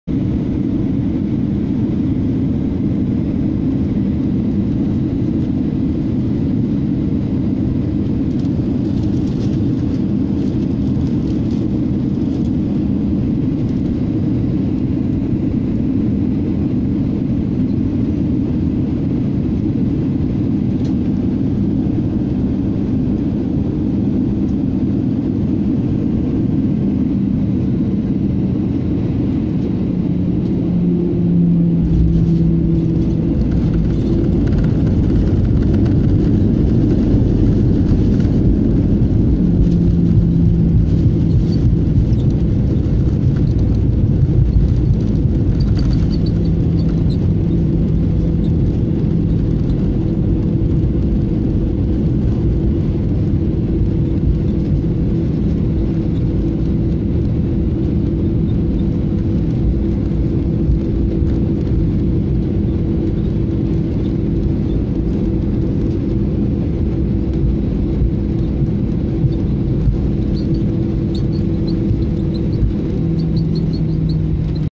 Landung in Hamburg (HAM) auf Landebahn 05, Germanwings Airbus A320 (D-AIPW)
Germanwings Flug 4U32 von Köln zum Flughafen Hamburg Fuhlsbüttel
Landeanflug in nordöstlicher Richtung auf Landebahn 05
Blick nach rechts (Süden)

Bei dem verwendeten Link handelt es sich um einen Affilate Link. Durch einen Kauf über den Link werde ich am Umsatz beteiligt. Dies hat keinen Einfluß auf den zu zahlenden Preis.